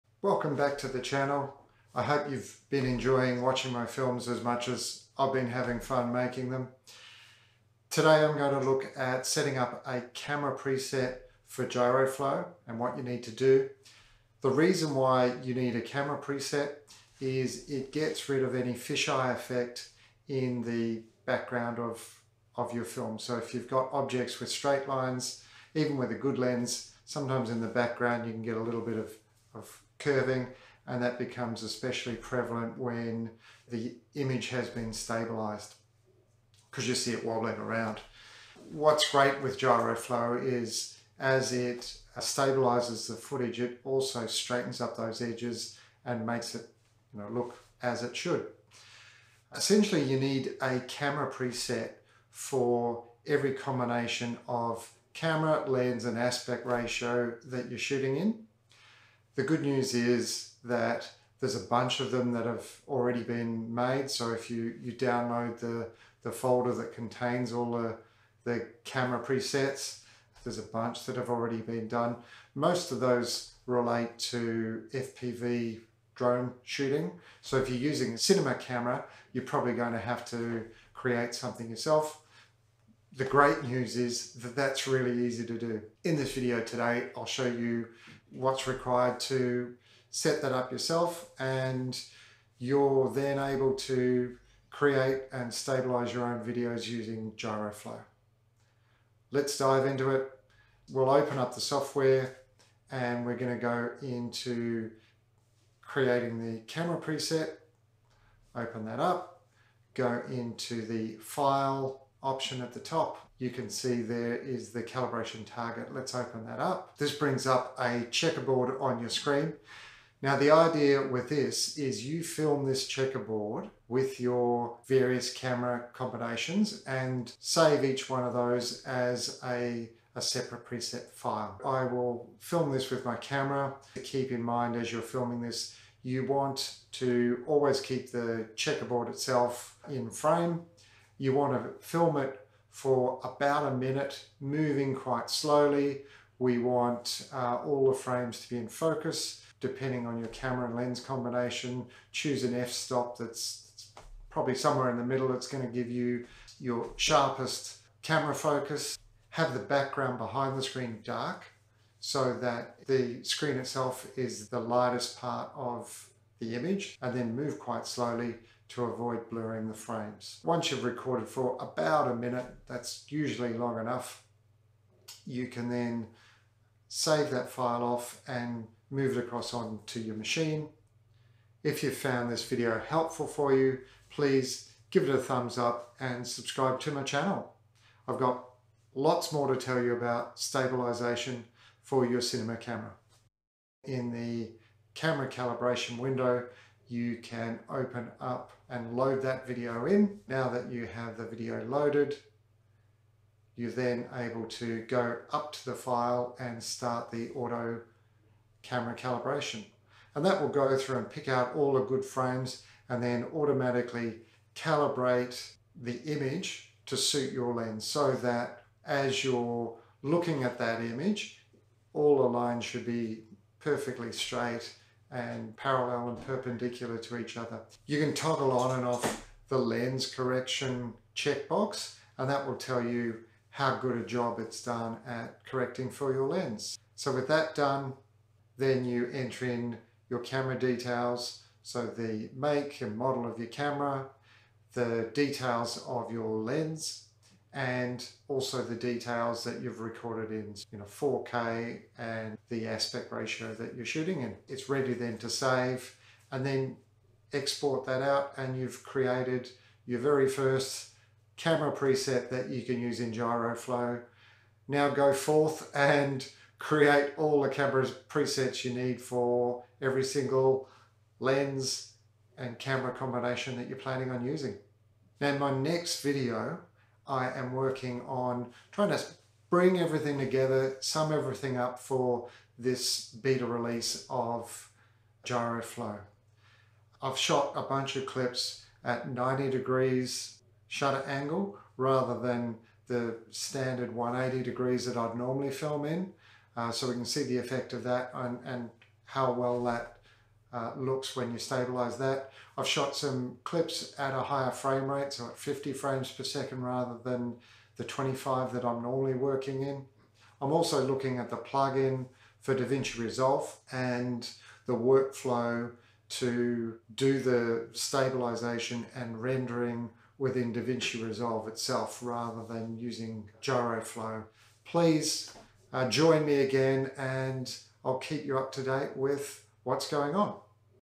Setting up your Camera Presets in Gyroflow
If you've decided to give Gyroflow a go, you'll need to either find an existing camera preset that matches your camera, lens and frame aspect ratio or make one yourself.  In this video, we'll go through all the steps required to make a camera preset for Gyroflow using it's in built auto calibration tool set.  It is simple to do and you'll be churning out camera presets for Gyroflow before you know it.